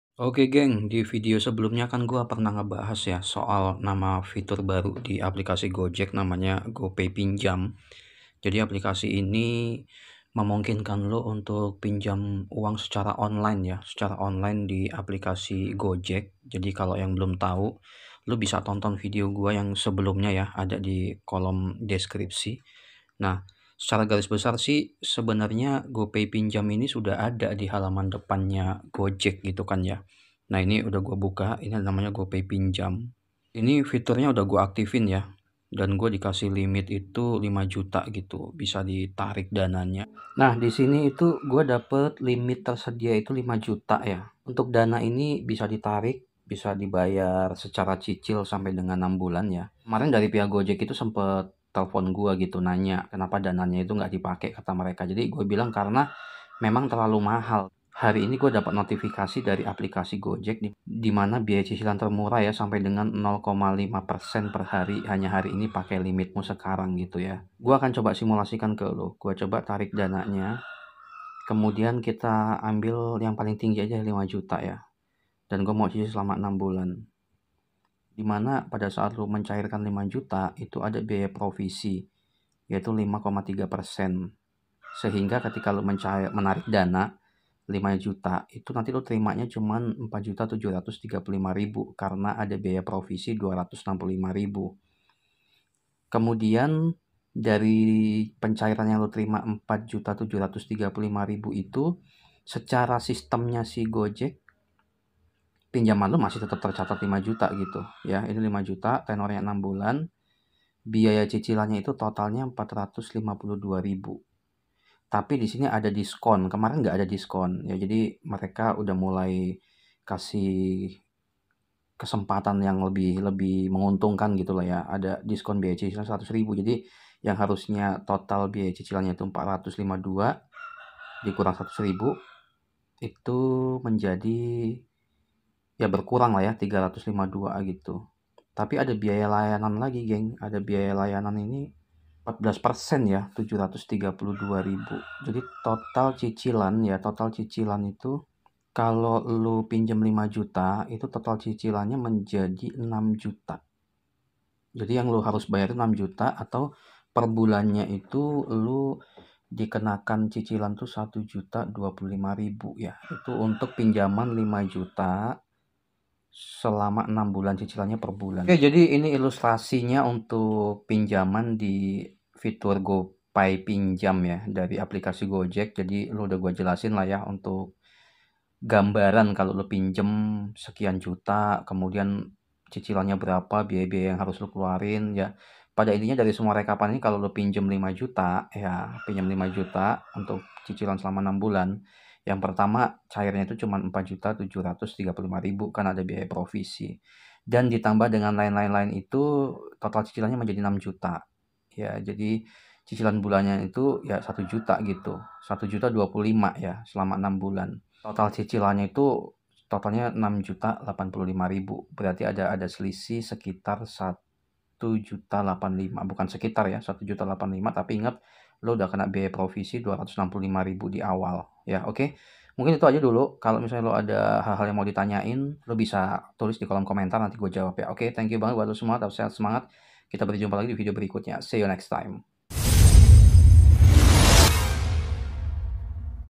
Cara Cepat Pinjam Uang Di Aplikasi Gojek Lebih Murah Dengan Penawaran Promo
Saat ini Aplikasi Gojek menawarkan fitur baru yaitu Gopay Pinjam.

Dengan adanya Gopay Pinjam di Aplikasi Gojek anda bisa menarik dana dari Aplikasi Gojek dan sistem pembayaran bisa di cicil.

Dana yang bisa di tarik dari Aplikasi Gojek cukup besar dan cicilan juga saat ini bisa sampai enam bulan.

Pada video kali ini, saya akan jelaskan mengenai Cara Cepat Pinjam Uang Di Aplikasi Gojek Lebih Murah Dengan Penawaran Promo

Terimakasih atas dukungan Anda